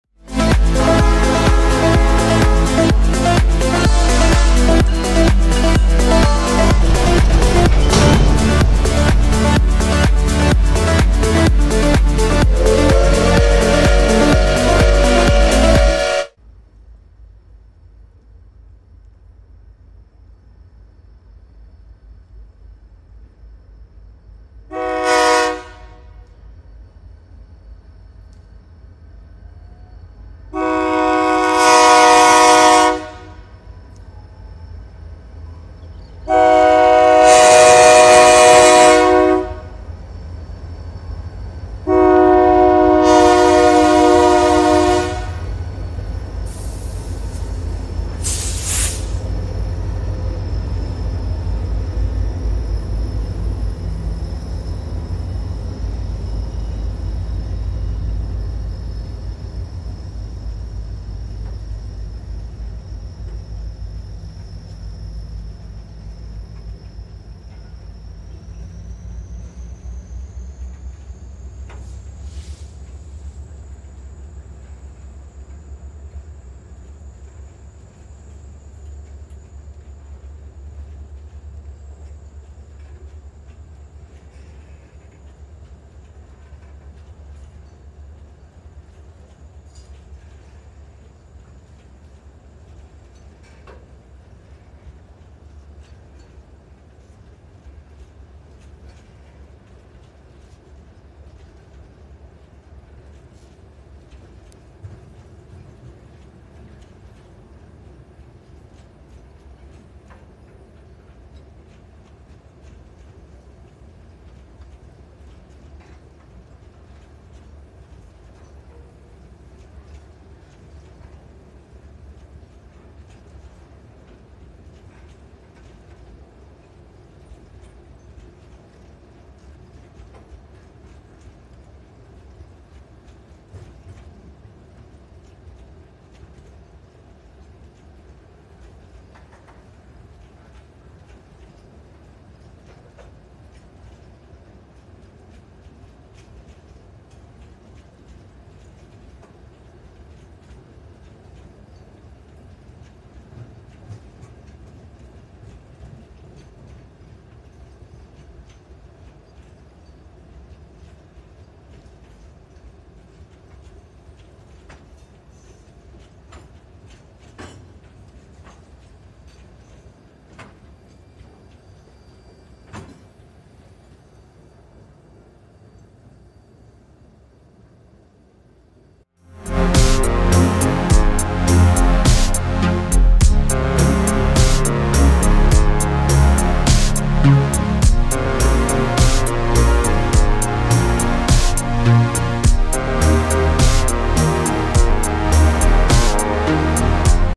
Para RGS com gondolas passando por Mafra - To RGS with gondolas passing through Mafra
Camera 2 - Esse trem esta abandonado no RGS pelo q estou sabendo, desceu mais nunca descarregou.